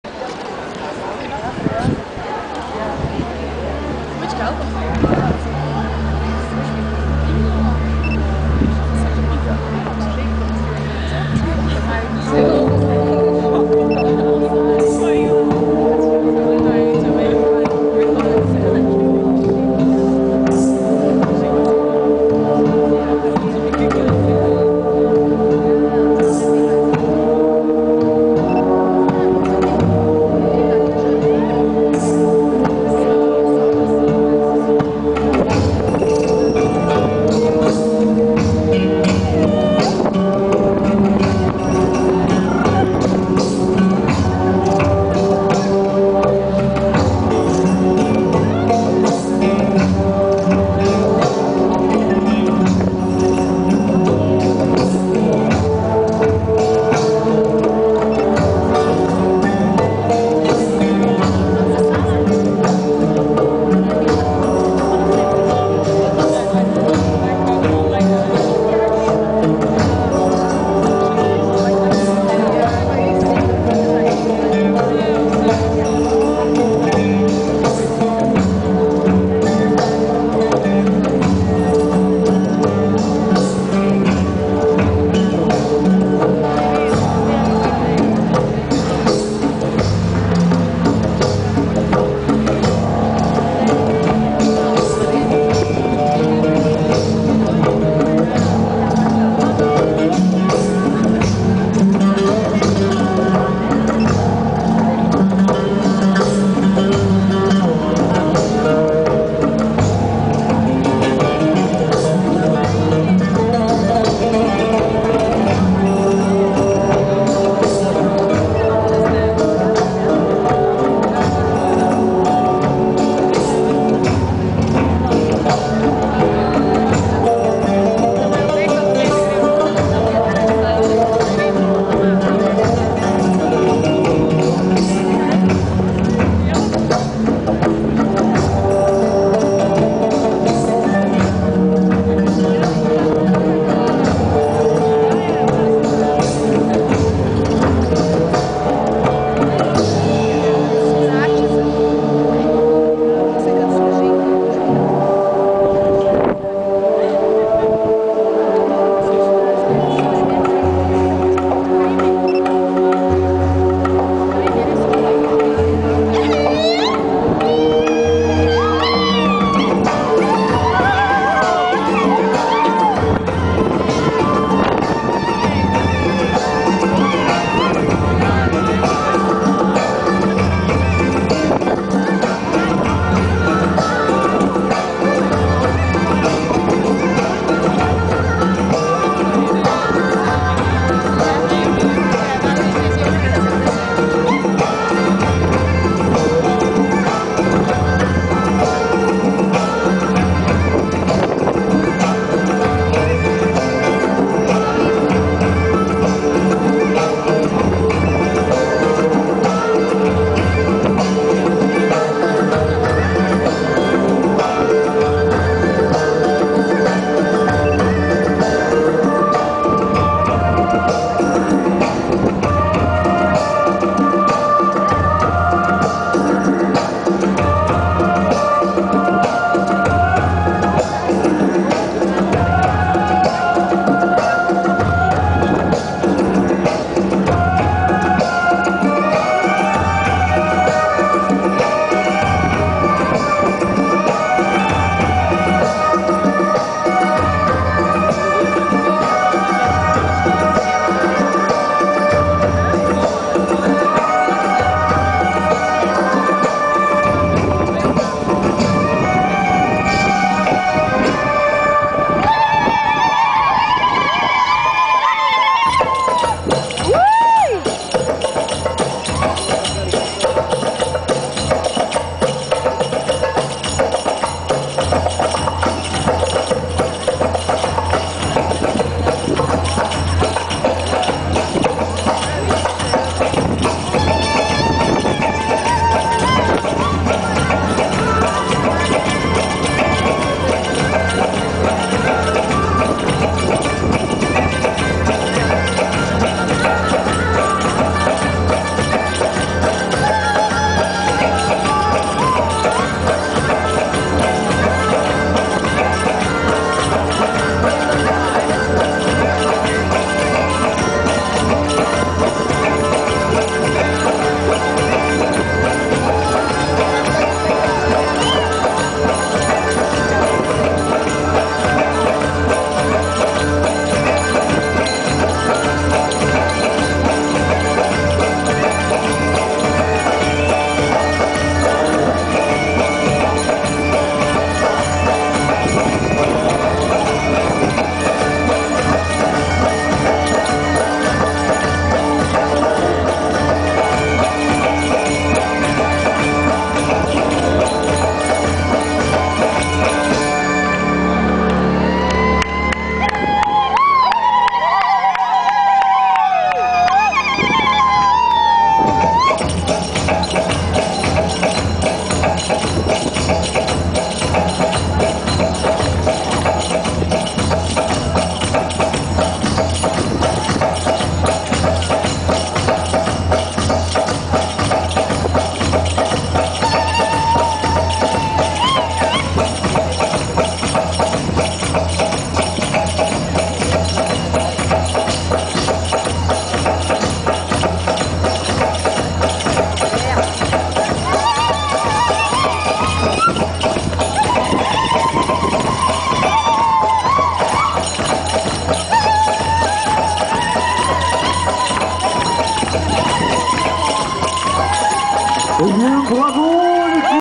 Improvisation en tribal ATS - Tribal Umrah 2009
Tribal ATS by Urban Shamans, directed by Julie de St Blanquat (Teacher @ Tribal UMRAH festival - 2009)

Improvisation en danse orientale tribale ATS de la compagnie Urban Shamans dirigée par Julie de St Blanquat lors du spectacle Tribal Umrah (Biarritz - 2009).

Video : Sabrina Milan